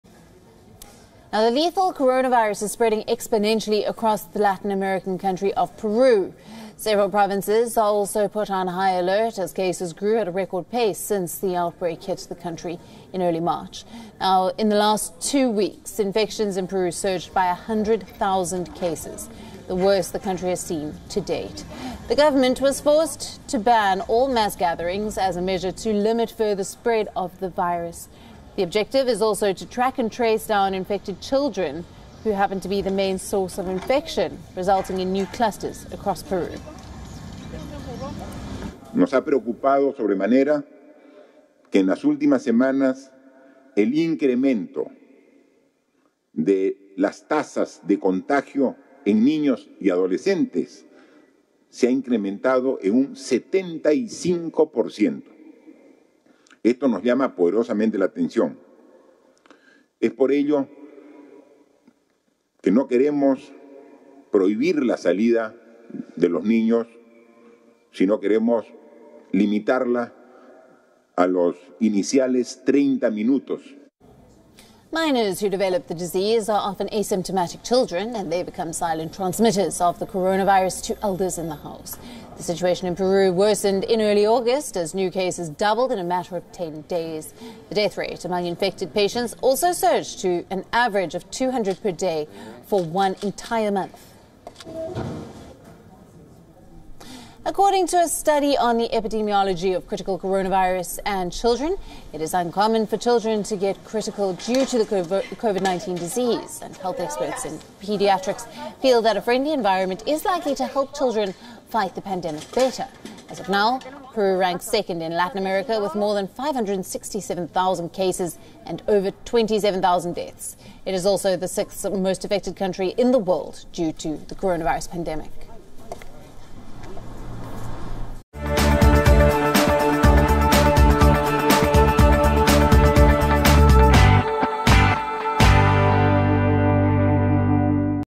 Coronavirus Outbreak: Peru reports 100,000 cases in two weeks | World News
In the last two weeks coronavirus infections in Peru grew by 100,000, the worst record since the disease broke out in the country in March, forcing the government to ban all family gatherings as a new measure to try to stop the pandemic.